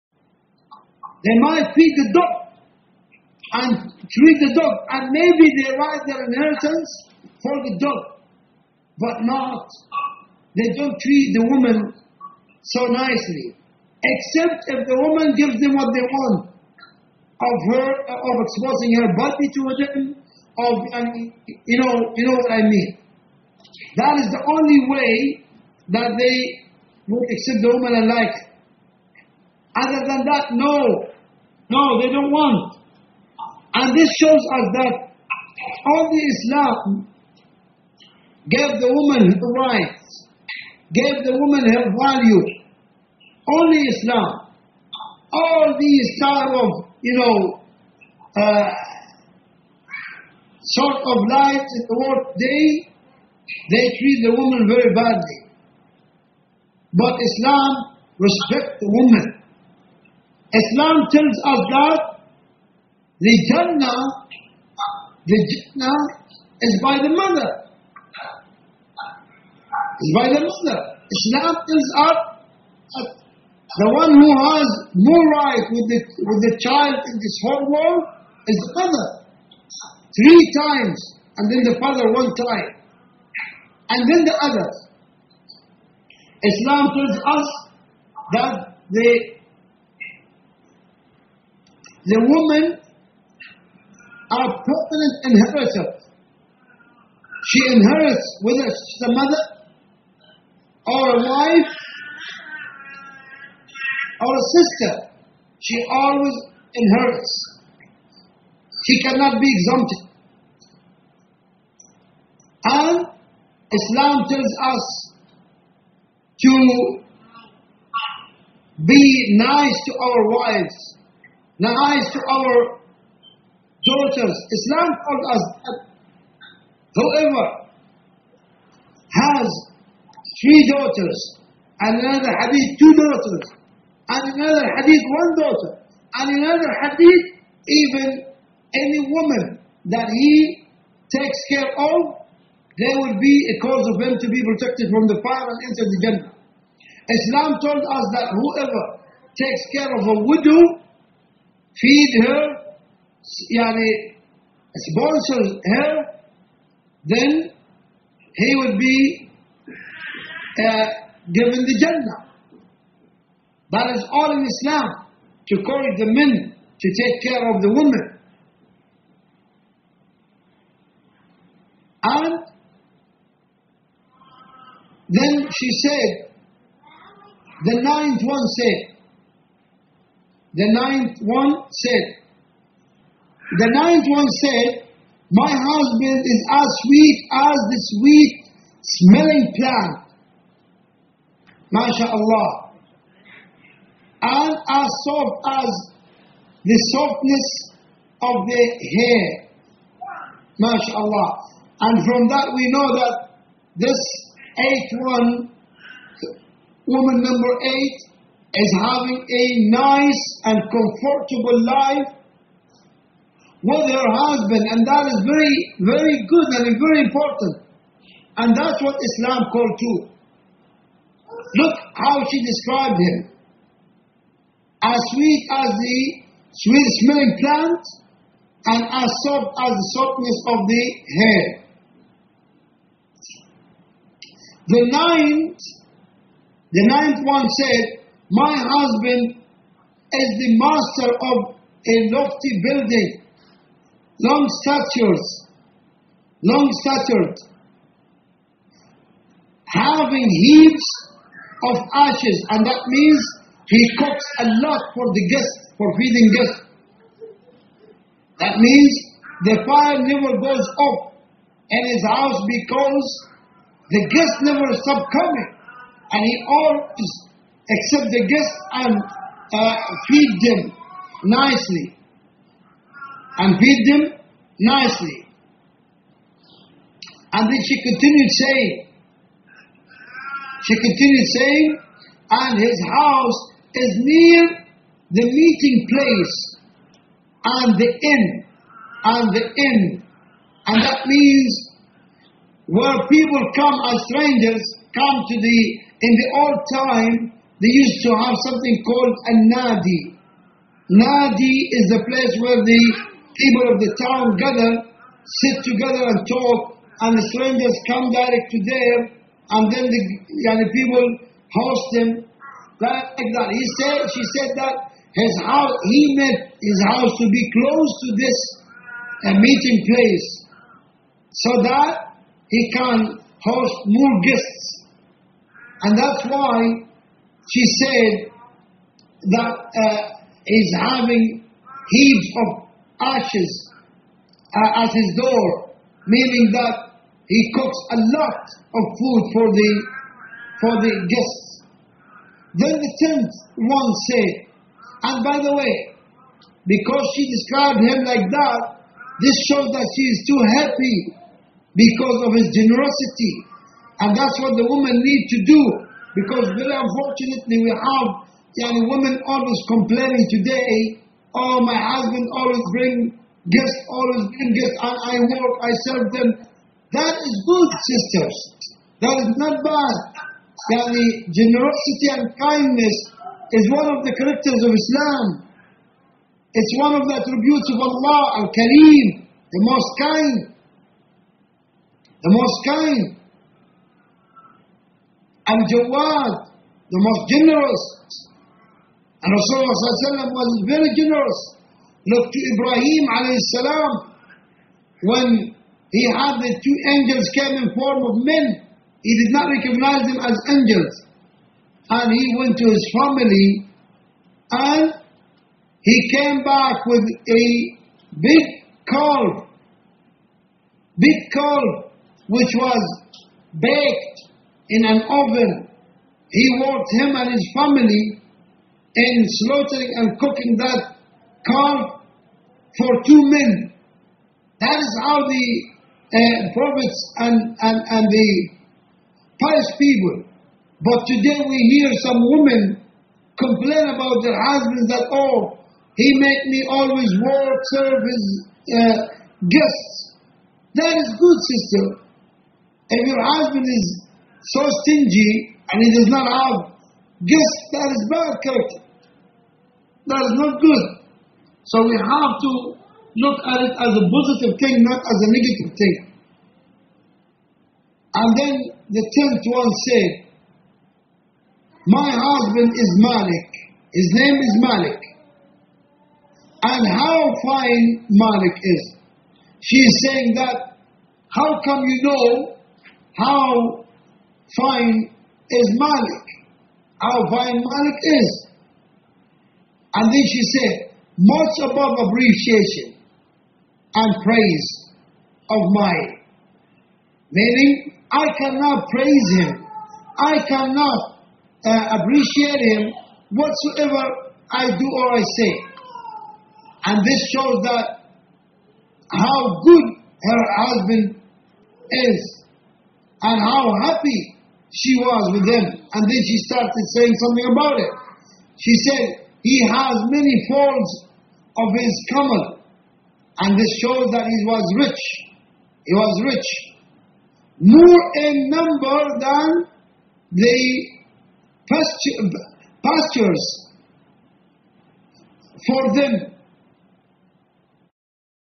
part 4/7 - Lessons from the Hadith of Umm Zarr
part 4/7 - Lessons from the Hadith of Umm Zarr - (Hadith of Eleven Women) by Shaykh Muhammad Al-Maliki

Presented by Sisters For Sisters - July 14th, Sunday, 2014